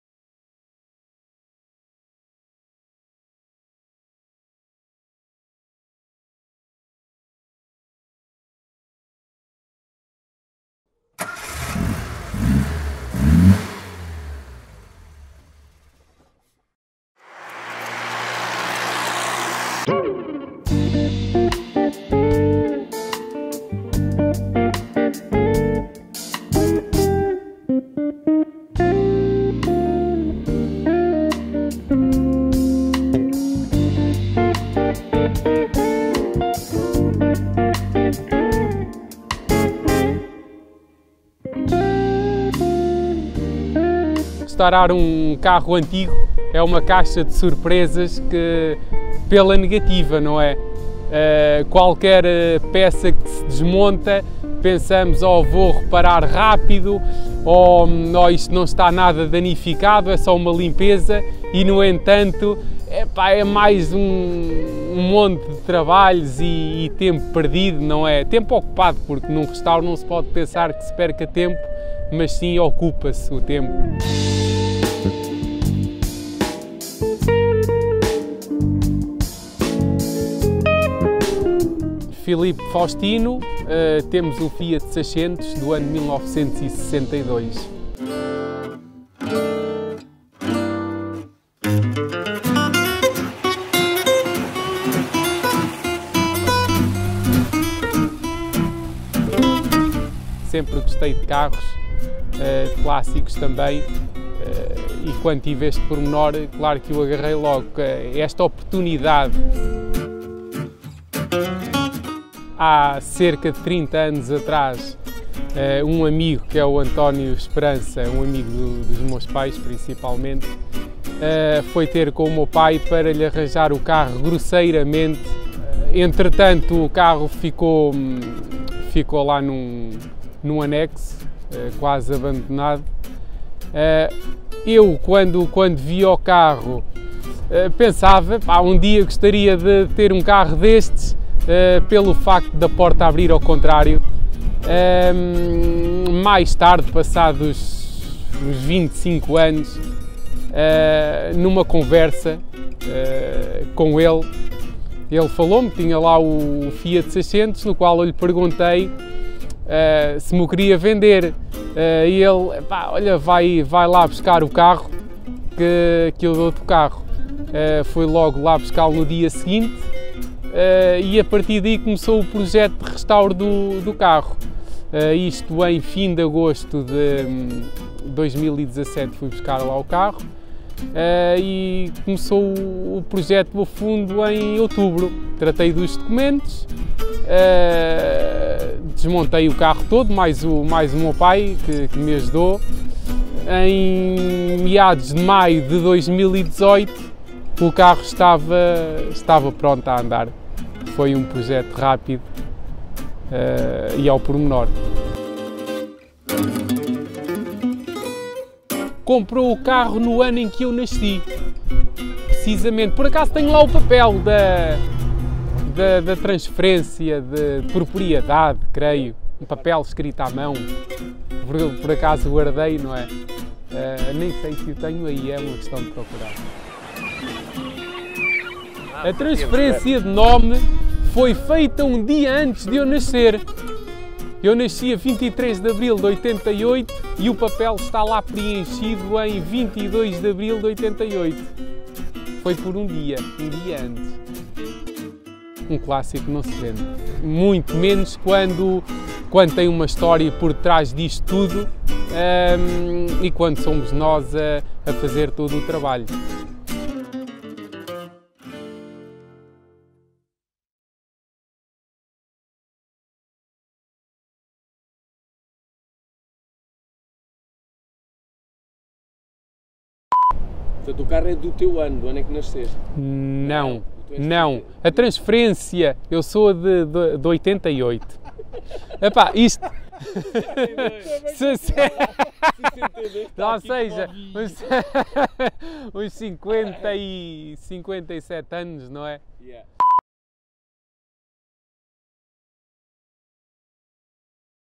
Fiat 600 D ENTREVISTA Ep.2.1 T.3 // Carburador
Neste video disponibilizamos toda a entrevista feita a Filipe Faustino, proprietário do Fiat 600 D. Fica a saber toda a história que está por trás deste clássico, desde a sua compra ao seu restauro.
Se gostaste deste video, faz like e partilha 👍